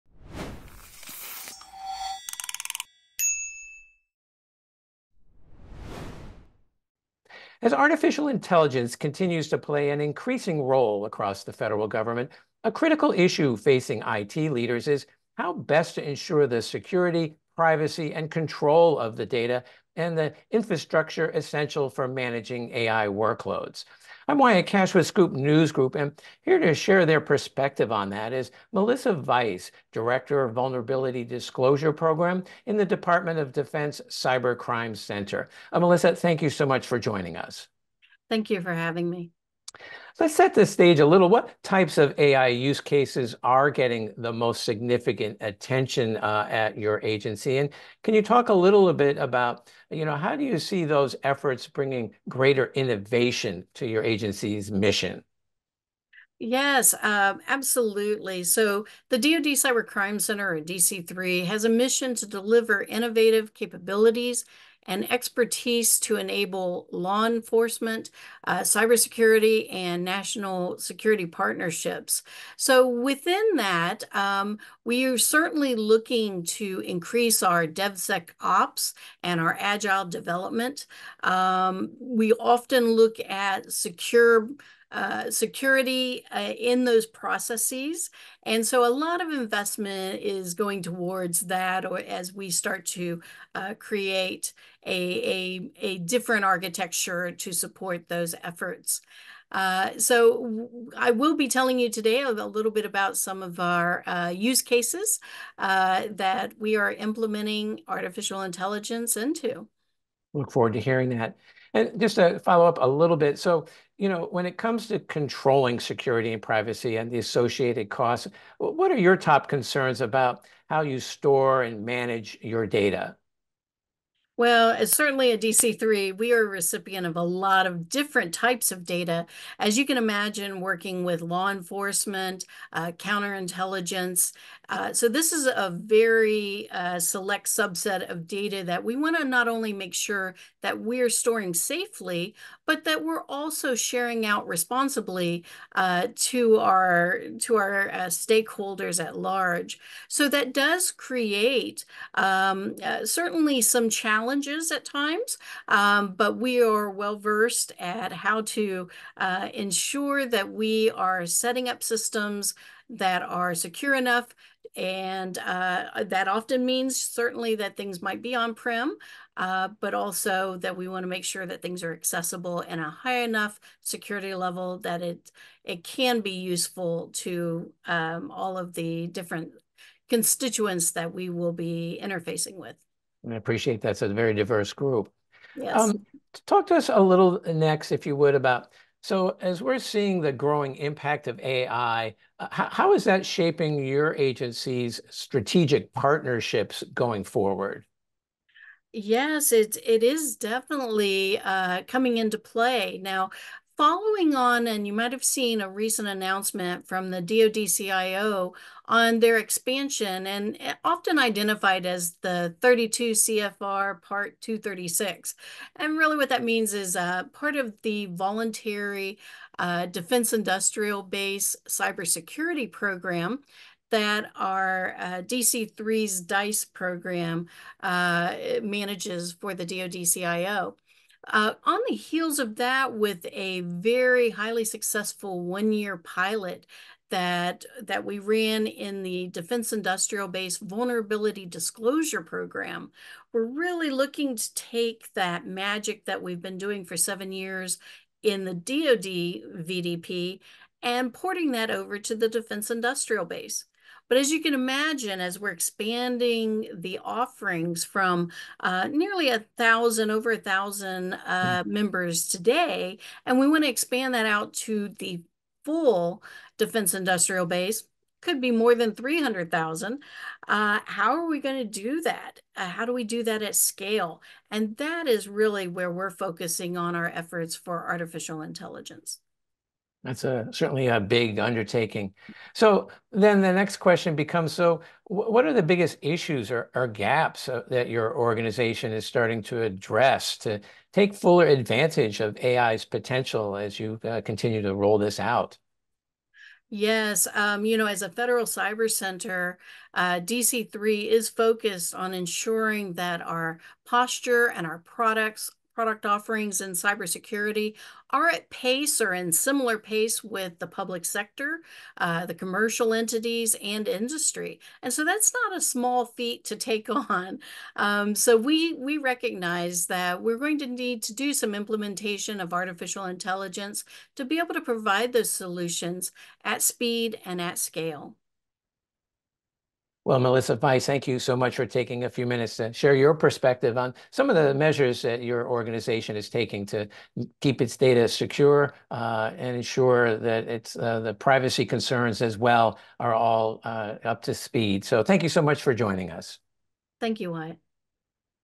Melissa Vice, DOD Cyber Crime Center | Advancing Government Innovation With Private AI
As artificial intelligence plays an increasing role across the federal government, a critical issue facing IT leaders is how best to ensure the security, privacy and control of data – as well as the infrastructure essential for handling AI workloads. Melissa Vice, Vice Director of the Vulnerability Disclosure Program in DOD’s Cyber Crime Center, discusses top concerns when it comes to storing and managing data.